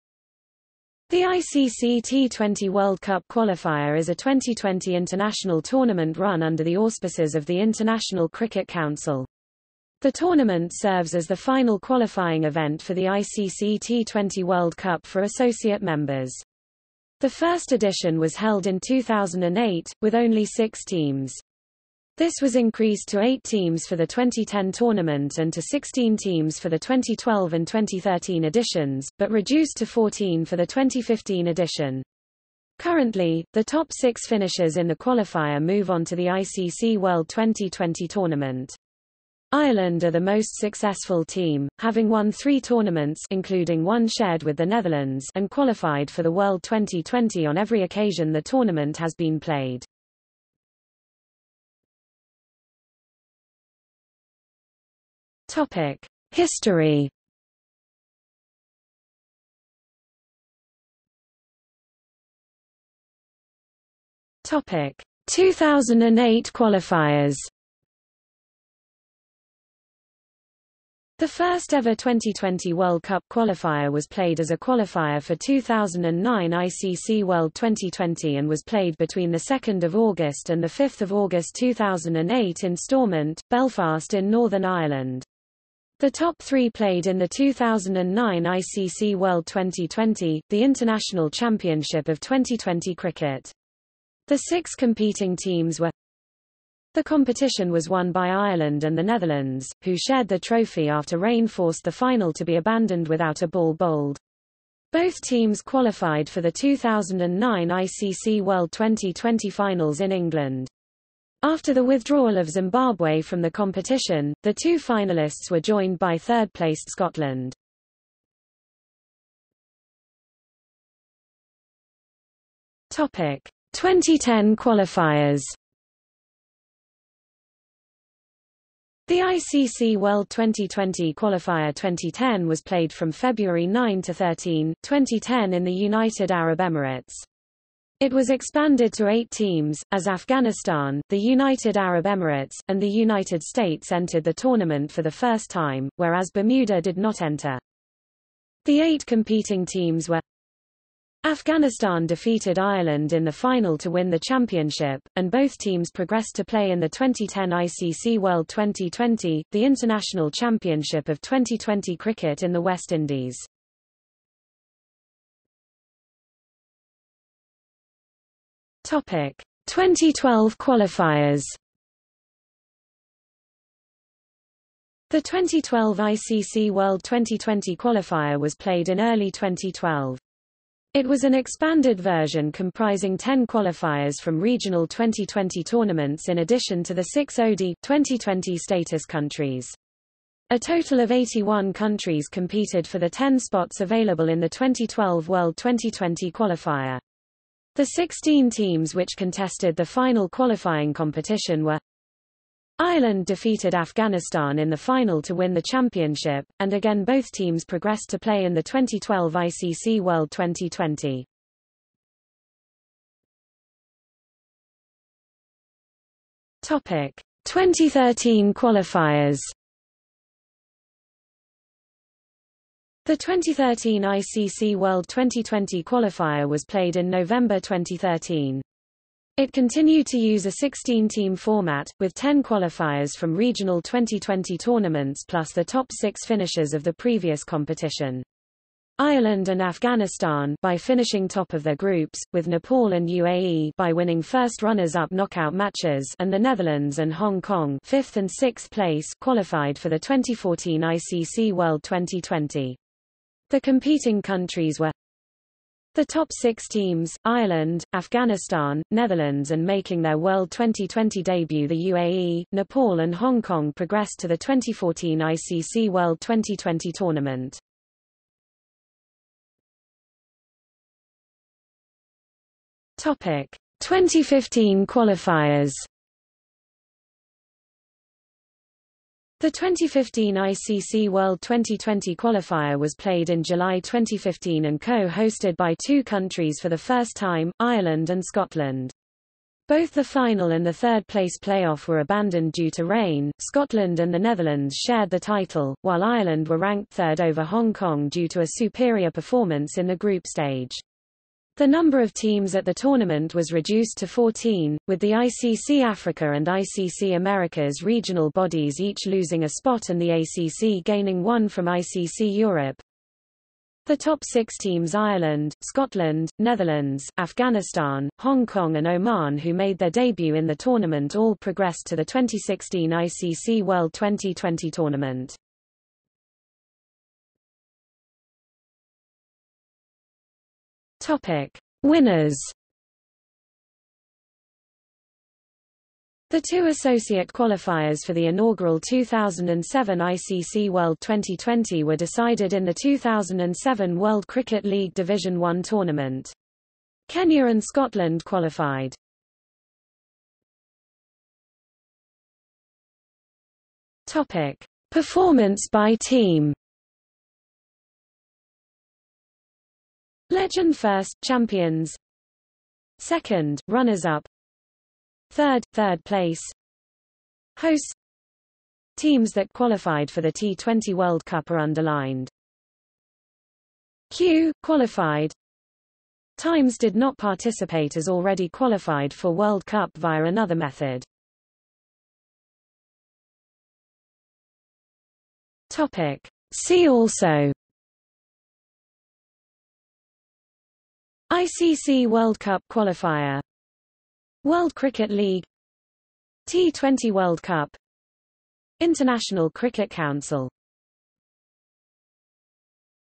ICC World Twenty20 Qualifier | Wikipedia audio article
00:00:51 1 History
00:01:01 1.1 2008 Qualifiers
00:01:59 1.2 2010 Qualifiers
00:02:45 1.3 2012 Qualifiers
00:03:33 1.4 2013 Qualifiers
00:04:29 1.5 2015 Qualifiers
00:05:27 2 Winners
00:05:50 3 Performance by team
00:06:22 4 See also

- Socrates

SUMMARY
The ICC T20 World Cup Qualifier is a Twenty20 International tournament run under the auspices of the International Cricket Council. The tournament serves as the final qualifying event for the ICC T20 World Cup for Associate Members. The first edition was held in 2008, with only six teams. This was increased to eight teams for the 2010 tournament and to 16 teams for the 2012 and 2013 editions, but reduced to 14 for the 2015 edition. Currently, the top six finishers in the qualifier move on to the ICC World Twenty20 tournament. Ireland are the most successful team, having won three tournaments (including one shared with the Netherlands) and qualified for the World Twenty20 on every occasion the tournament has been played.